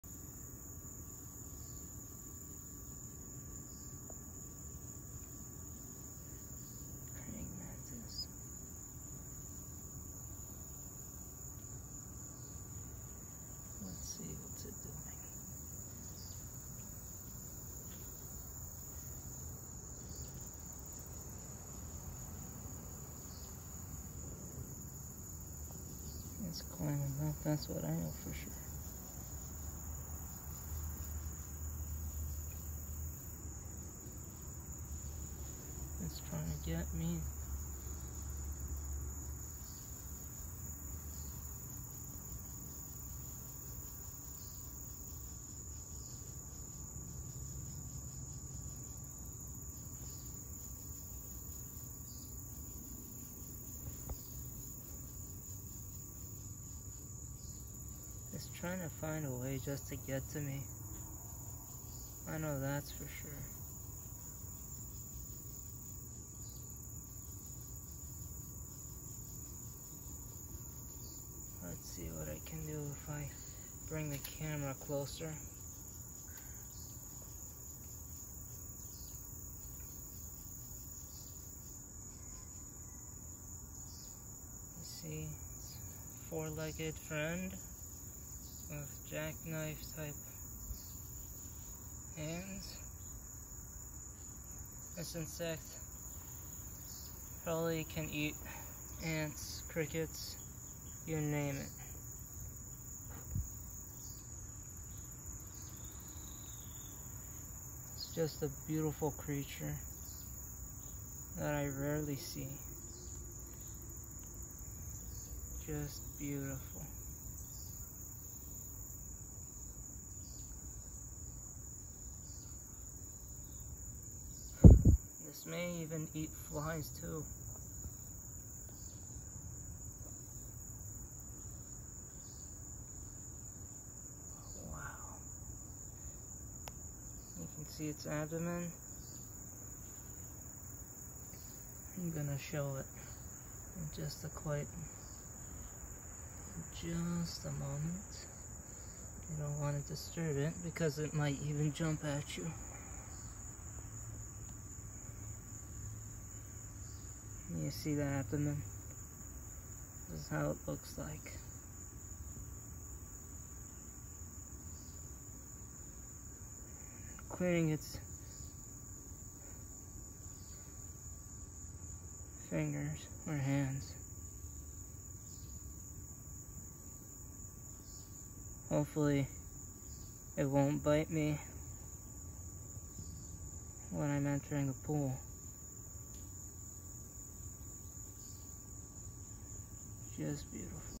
A preying mantis spotted
It was just a typical day outside before I could go out swimming outside in the pool at a friend’s house until I noticed a mantis just rarely appear near the pool at a friend’s house. As I got curious to know what such a bug is able to do, what got my attention most was the mantis’s body size and structure. What do you think about mantis’s? Leave your thoughts on the comments section below.